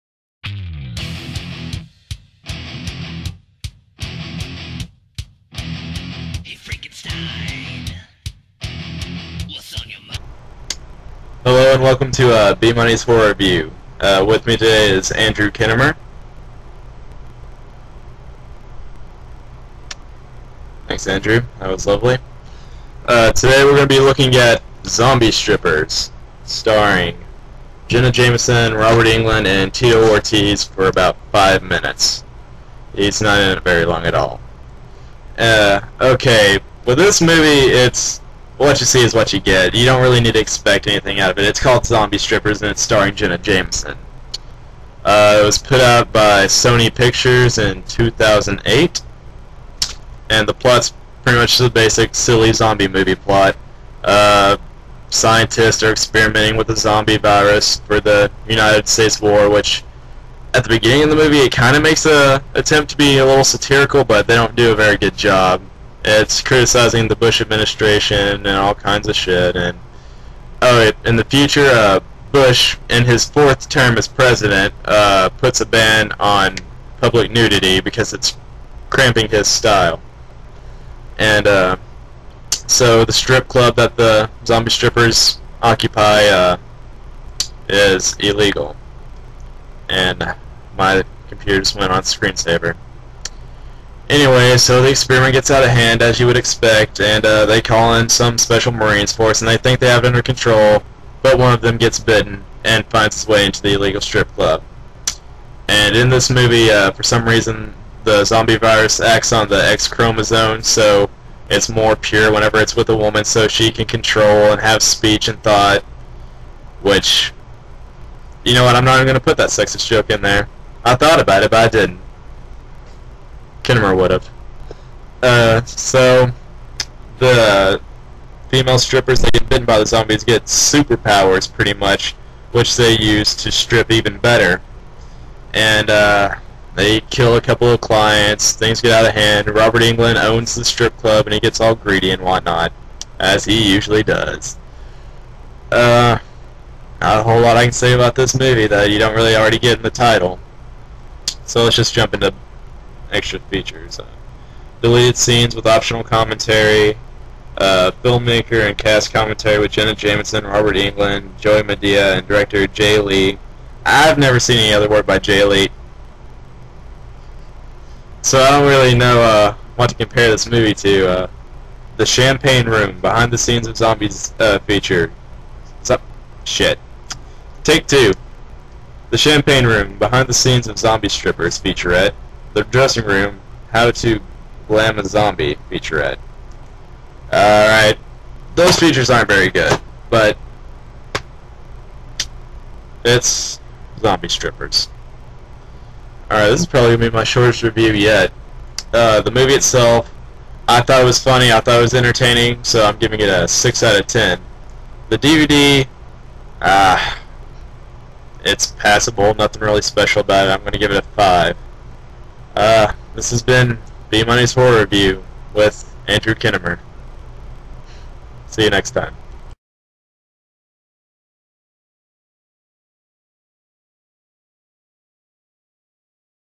B-Money's Horror Review Episode Six: Zombie Strippers
Zombie Strippers(2008) Sony Pictures
Robert Englund(Nightmare on Elm Street) Jenna Jameson(porn. )
Movie Score 6/10 DVD Score 5/10